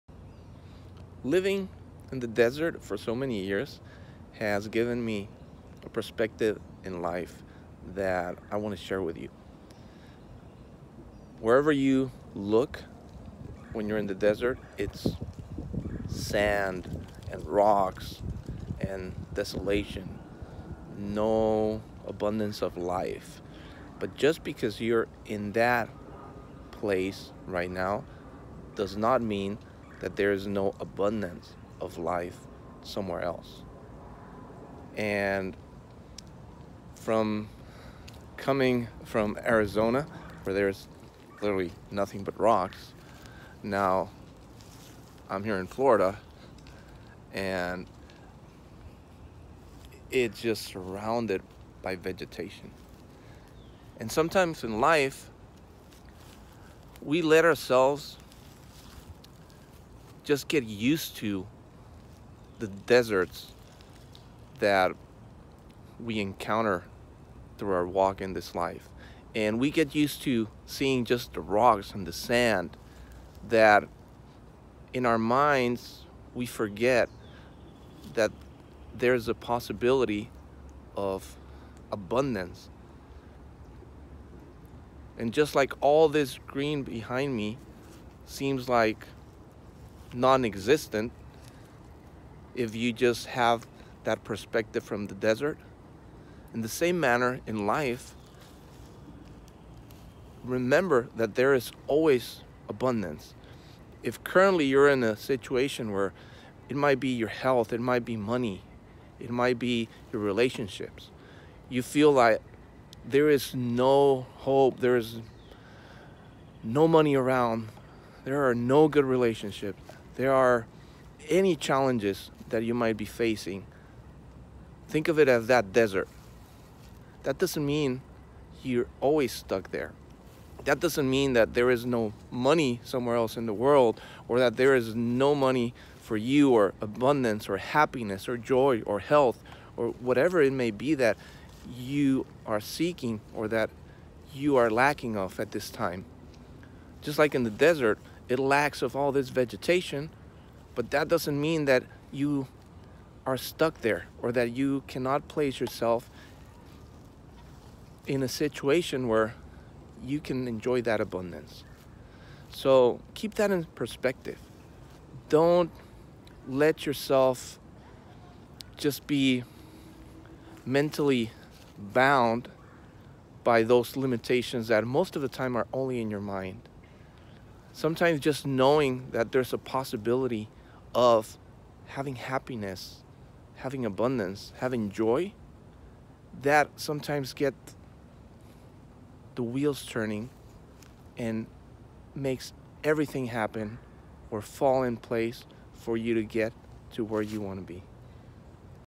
Being in a desert doesn’t mean that lush vegetation does not exist.
We are conditioned to measure things based on what surrounds us. If you are currently in the ‘desert’ you may think or believe that sand and rocks are the only thing available in life for you. However, that doesn’t have to be true. There is vegetation and abundance and you can get yourself there if you want. You have to start by knowing that this is posible and get out of that tunnel vision mindset that may be holding you back. Joy, health, happiness, peace, and love are a reality that you can experience.